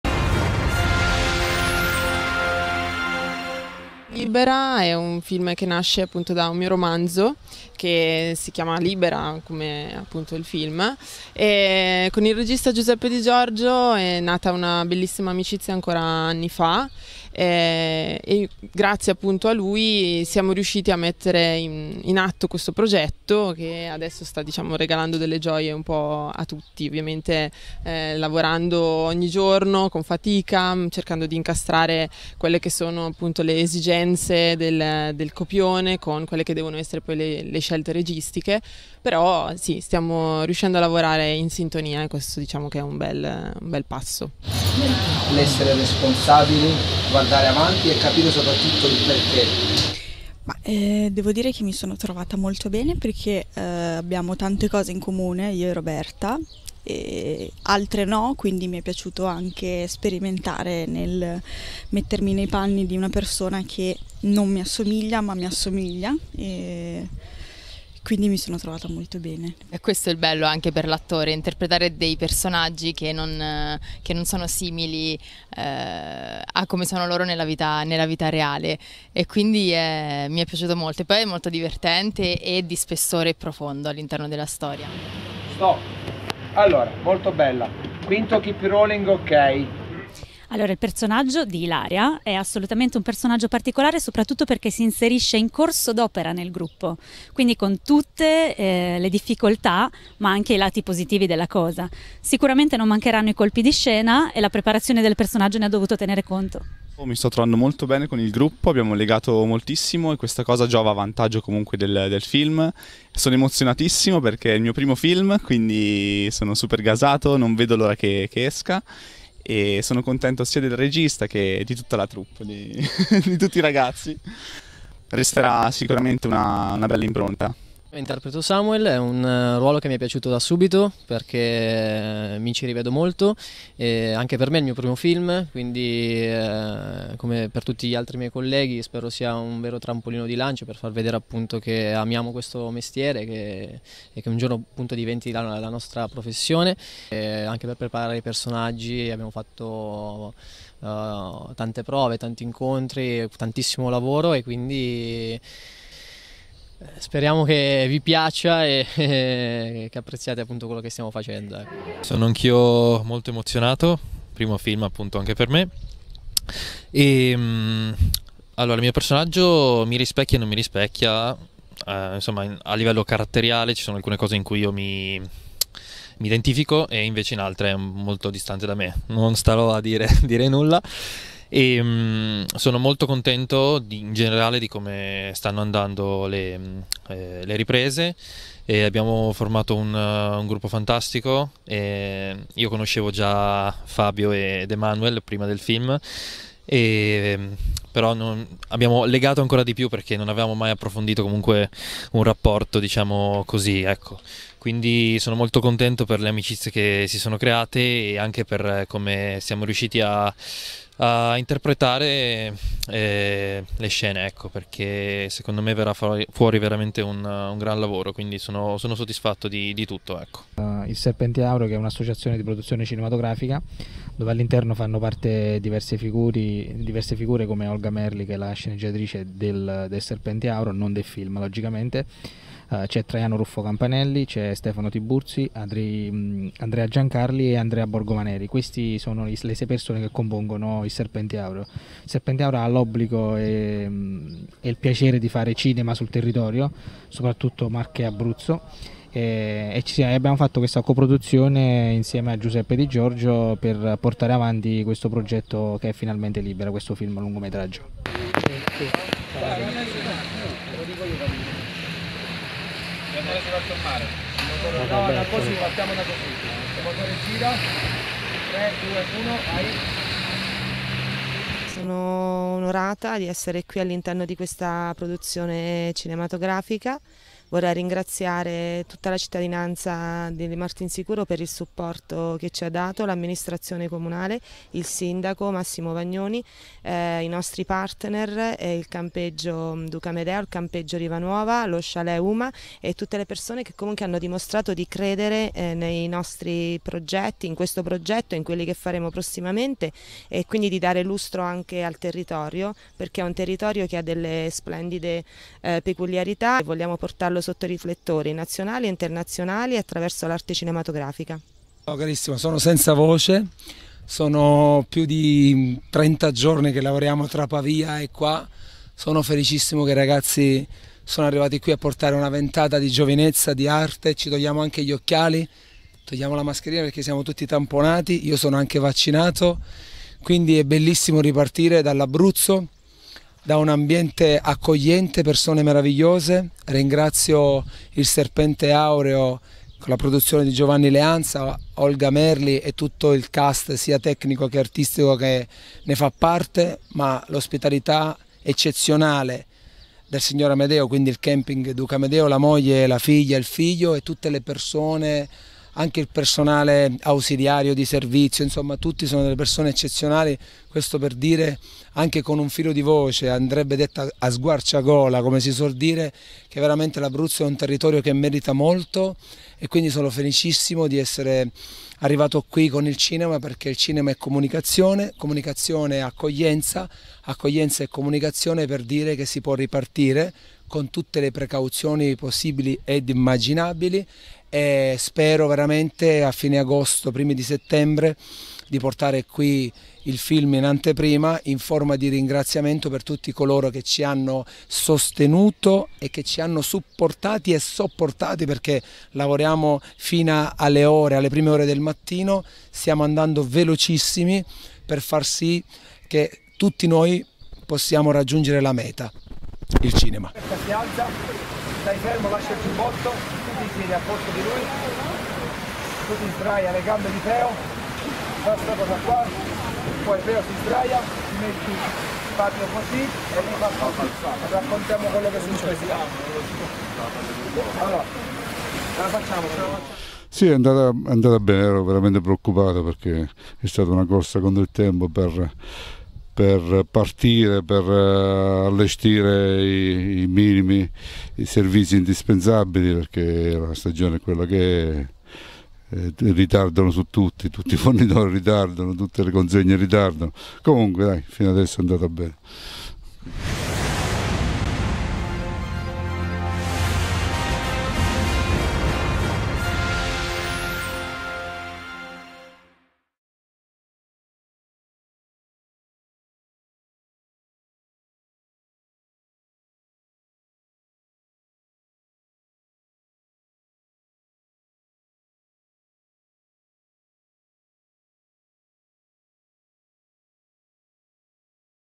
intervista al Cast “finalmente Libera”
film di Giuseppe Di Giorgio “Finalmente libera”, ispirato al libro di Gabriella Morgillo, che ne è anche la sceneggiatrice, coprodotto da “Il Serpente Aureo” di San Benedetto Del Tronto e da Elena Rivaldo. Siamo andati sul set del film e abbiamo parlato con il regista, con la sceneggiatrice, con l’attrice protagonista Camilla Tedeschi, con l’attrice ascolana Erika Franceschini, e con tre rappresentanti de “Il serpente Aureo”: Giovanni Leanza, Direttore di Produzione, Stefano Tiburzi, Produzione e Comunicazione, e Olga Merli, autrice e sceneggiatrice cine-televisiva di “Il Serpente Aureo” e docente dell’AIFAS di San Benedetto del Tronto (Accademia internazionale di arte e spettacolo) diretta da Marco Trionfante.